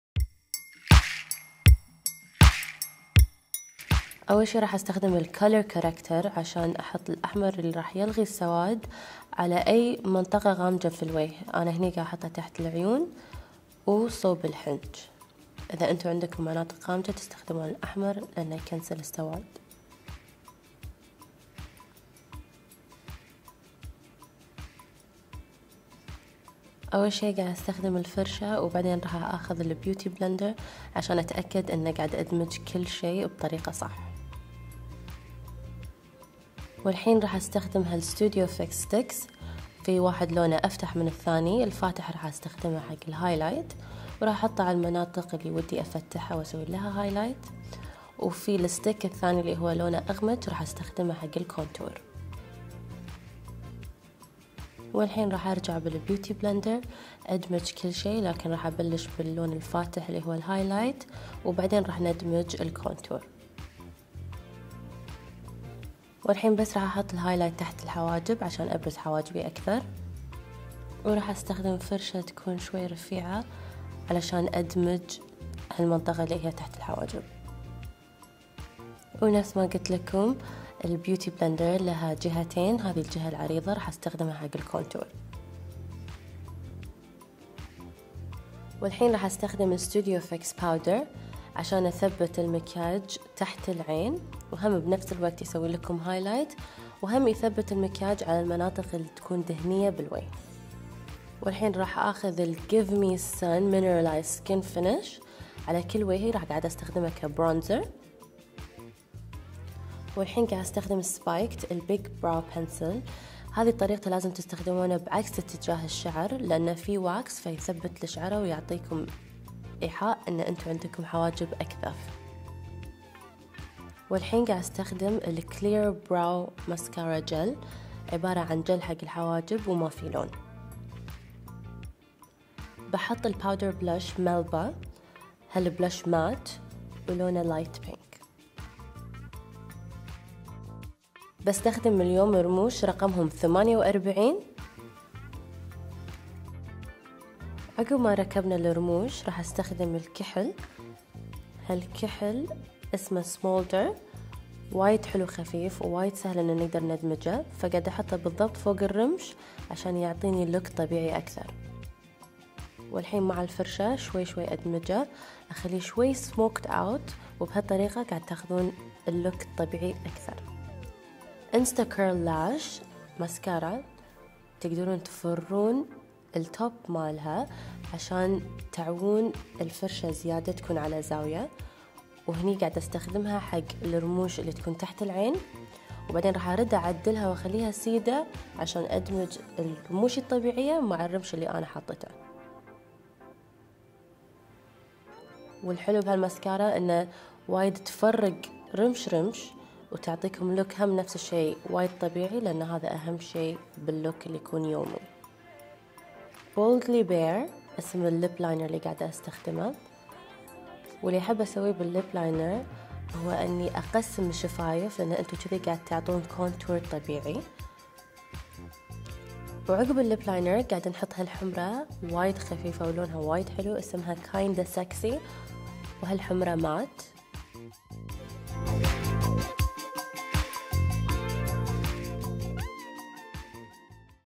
Daily Makeup Look By Fouz Al Fahad - مكياج يومي خفيف مع فوز الفهد
A radiant daily makeup look by Fouz Al Fahad in simple & easy steps!
!مكياج يومي ناعم مع فوز الفهد بخطوات سهلة
: اضغطي هنا لتسوق اطلالة فوز في السعودية
: اضغطي هنا لتسوق اطلالة فوز في الإمارات

Products Used - المنتجات
Studio Conceal & Correct Palette
Studio Fix Perfecting Stick
Studio Fix Perfecting Powder
Powder Blush in Melba
Mineralize Skinfinish Natural in Give Me Sun!
Clear Brow Gel
Big Brow Pencil in Spiked
Smolder Eye Kohl
48 Lashes
Instacurl Lash Mascara
Boldly Bare Lip Pencil
Kinda Sexy Lipstick